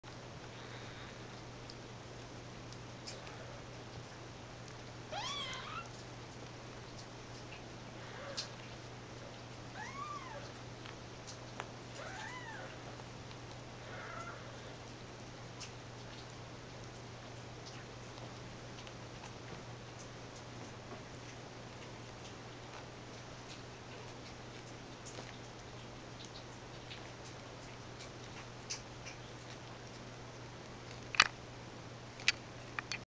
Foster kitten war!
Our two older foster kittens wrestling! Brutus and Runty are about 3.5 weeks old and come from the San Martin Animal Shelter in Santa Clara County. They are living with a foster Mommy cat and her 4 babies.